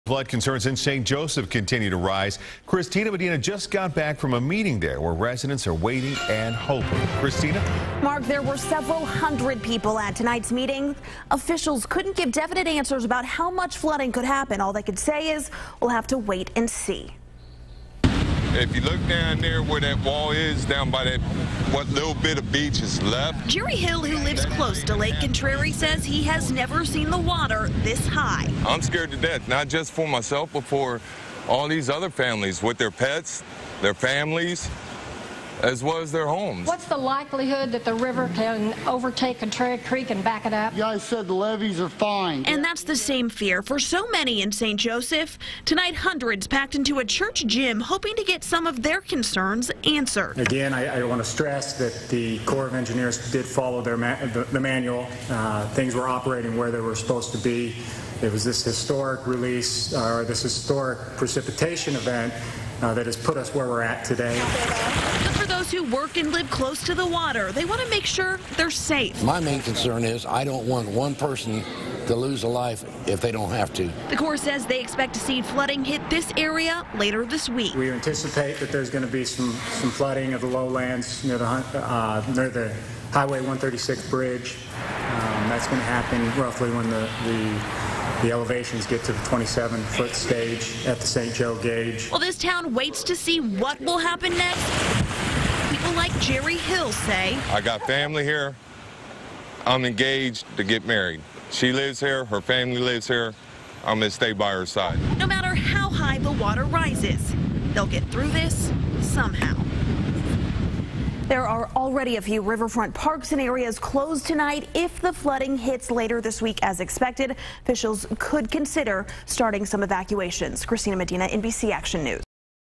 Flood concerns in St. Joe rise
Flood concerns in St. Joseph continue to rise.